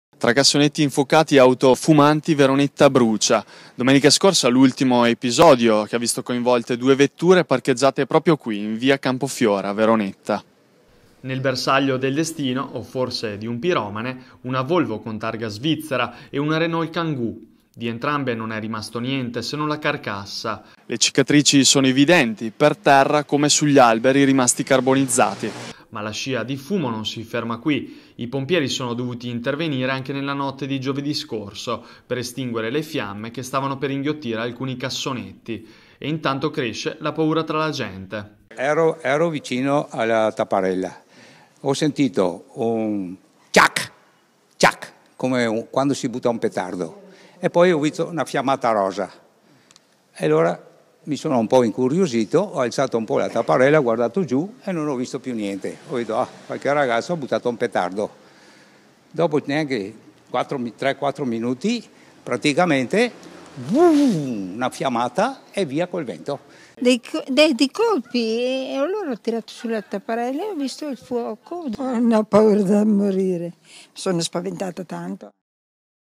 CHI C'È DIETRO GLI INCENDI DI VERONETTA?
Verona | È una settimana di fuoco per Veronetta. Più volte i pompieri sono intervenuti in via Campofiore: giovedì per estinguere le fiamme in alcuni cassonetti della zona e domenica per domare il fuoco che aveva inghiottito due vetture. Alcuni residenti ci hanno raccontato cos'hanno visto.